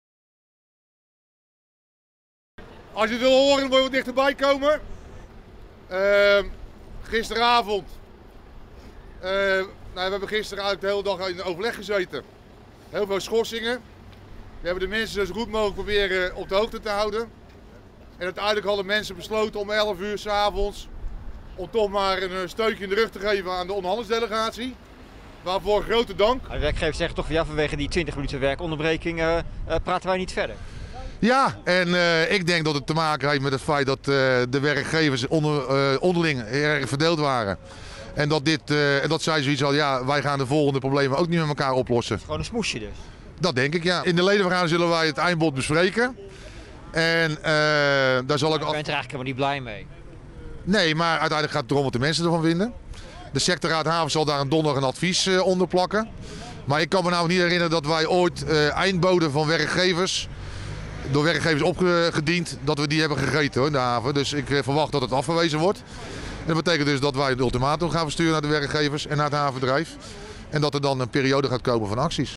FNV: 'Begin januari zal het raak zijn in de havens'
Medewerkers van de Rotterdamse haven leggen naar verwachting drie dagen in januari en mogelijk ook drie dagen in februari massaal het werk neer. "Ik ga ervan uit dat het begin januari raak zal zijn, want het huidige eindbod van de werkgevers zullen de havenwerkers niet slikken", zegt bestuurder Niek Stam van FNV Havens.Komende vrijdag stemmen de havenwerkers over het eindbod van de werkgevers. Waarschijnlijk is deze bijeenkomst in Ahoy Rotterdam of in voetbalstadion De Kuip. Het eerdere plan om ook drie dagen in december te staken is verschoven. Dit vanwege de naderende vakantieperiode.BlokkadeOp meerdere plekken in de Rotterdamse havens loopt het werk vertraging op als gevolg van stukgelopen onderhandelingen vrijdagavond. Zo blokkeerden ongeveer 200 werknemers korte tijd de poort bij de Delta-terminal van ECT. Ze werden voor de ingang bijgepraat door de vakbond. Stam: &quot;Elke werkploeg wordt geïnformeerd en dan besluiten ze zelf hoe laat ze gaan werken. Dat kan voor vertragingen zorgen.&quot;Spontane actiesHet overleg tussen de vakbonden, werkgevers en het Havenbedrijf Rotterdam liep vrijdagavond stuk. Spontane stakingen bij bedrijven als ECT, Euromax, APMT en APMT 2 waren reden voor de werkgevers om het overleg gelijk te beëindigen.Het Havenbedrijf Rotterdam heeft een werkgelegenheidsgarantie van 4,5 jaar aangeboden. Samen met de werkgevers hebben ze een pakket van maatregelen opgesteld om te voorkomen dat werknemers banen verliezen de komende jaren.Niet voldoendeVolgens vakbond FNV heeft het overleg vrijdagavond toch te weinig opgeleverd. De medewerkers eisen betere maatregelen om hun baan te behouden de komende jaren. De bonden zijn bang dat door automatisering van de terminals de komende jaren honderden banen verloren gaan.[Tweet